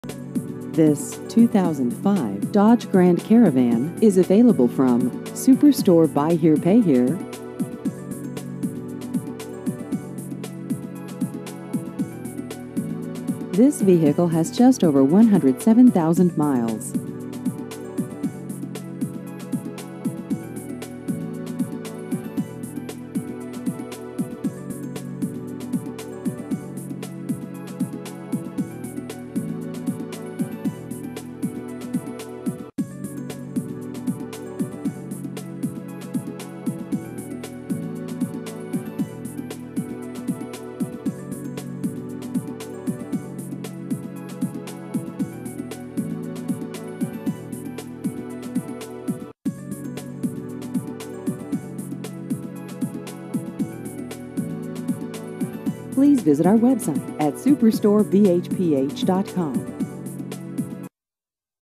2005 Dodge Grand Caravan available from Super Store Buy Here
This 2005 Dodge Grand Caravan is available from Super Store Buy Here Pay Here.